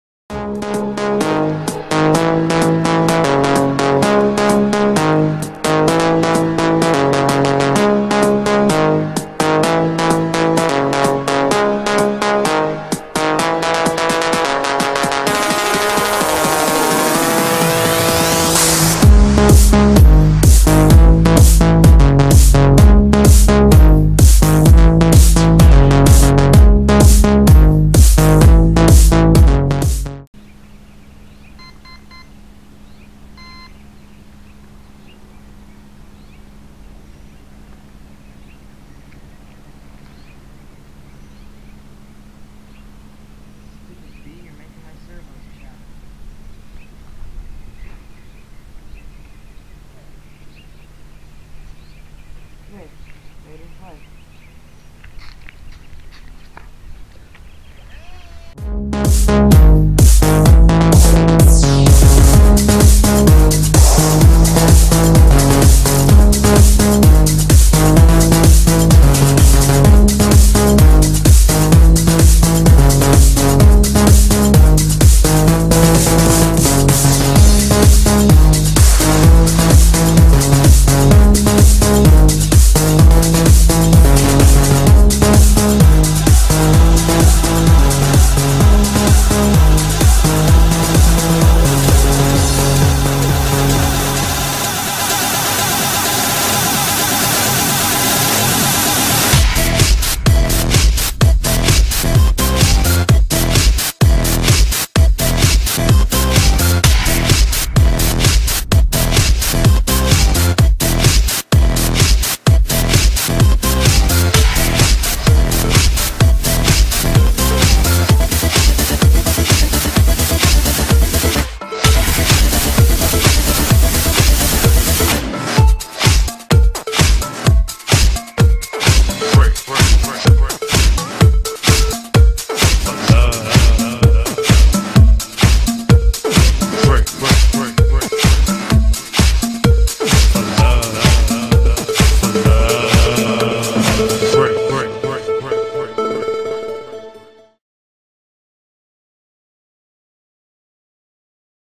Grim Reaper RC flying wing maiden flight
just a fast edit of my crash test hobby grim reaper my new fpv plane